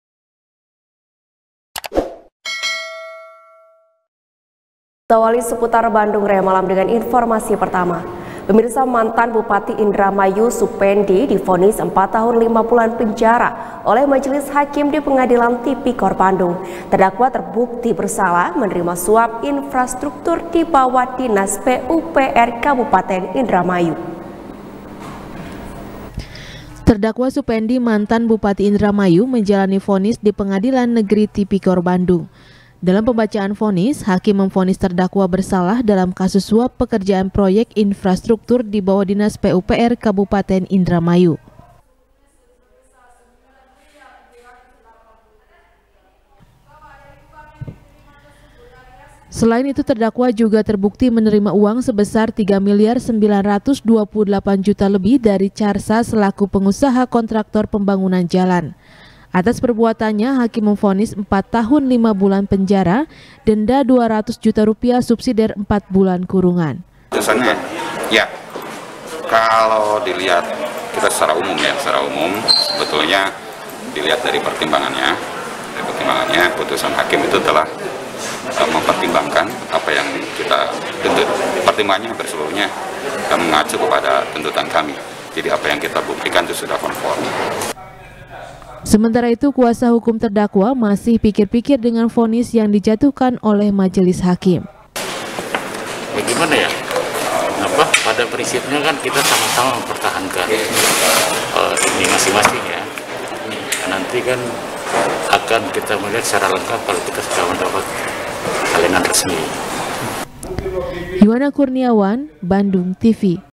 Mantan Bupati Indramayu Divonis 4 Tahun 5 Bulan Penjara | SBR | BANDUNGTV
SBR | BANDUNGTV | HANY SHANDRA | Mantan Bupati Indramayu, Supendi,  divonis empat tahun lima bulan penjara oleh majelis hakim di Pengadilan Tipikor Bandung. Terdakwa terbukti bersalah menerima suap infrastruktur di bawah Dinas PUPR Kabupaten Indramayu.